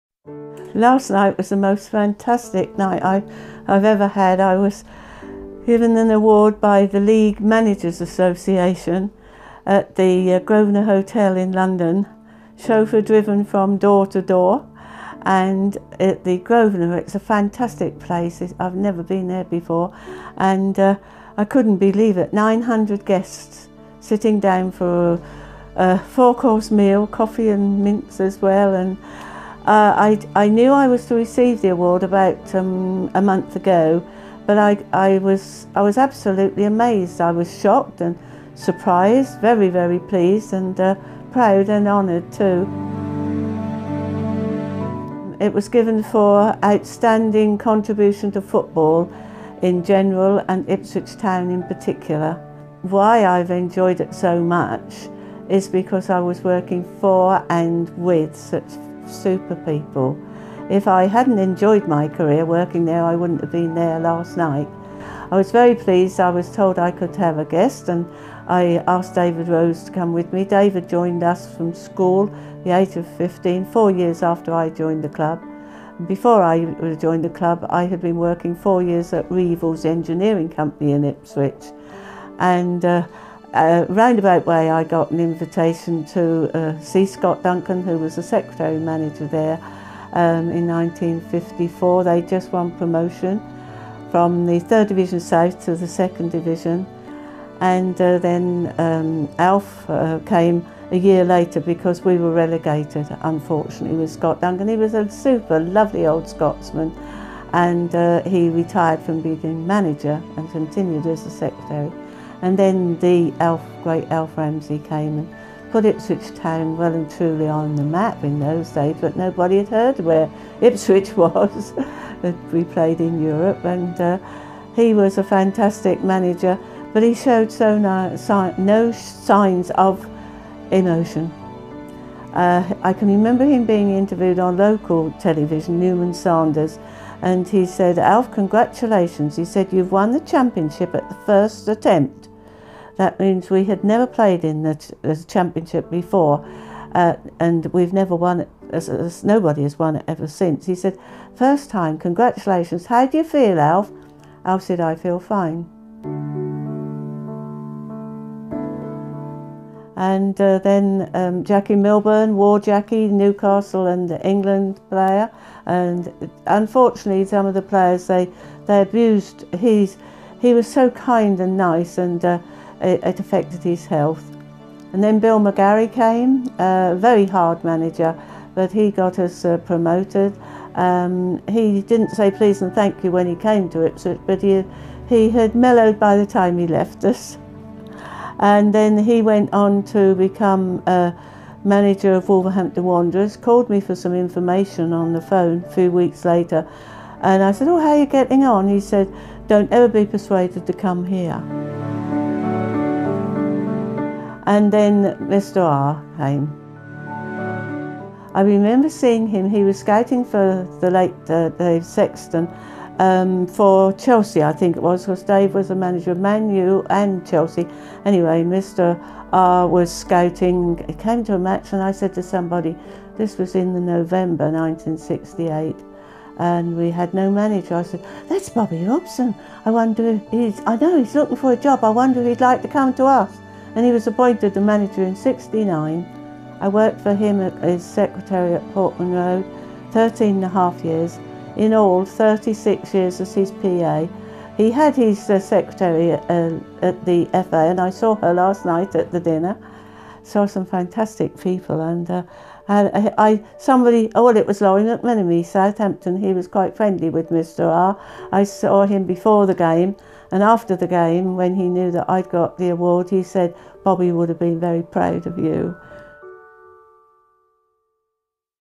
Meet the woman 'married' to Ipswich Town Football Club
Pat Godbold has worked at Ipswich Town FC for 65 years. This week she was given a special award by the League Managers Association in recognition for her service to football.